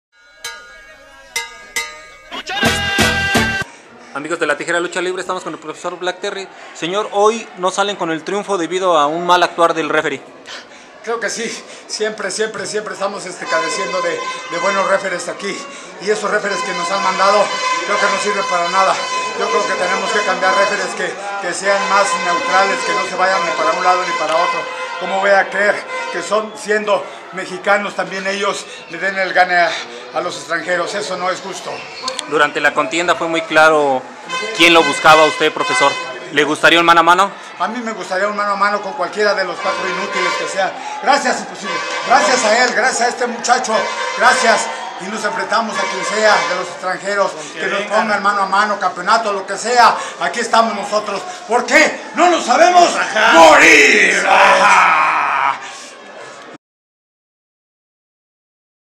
Black Terry: "Estos referís no sirven para nada; gracias Imposible por tu apoyo"
El profesor Black Terry nos habla que están cansado de los malos referís de la Arena Naucalpan y de Hip Hop Man, al que ha visto que habla mucho y lucha poco; agradece el apoyo del Imposible...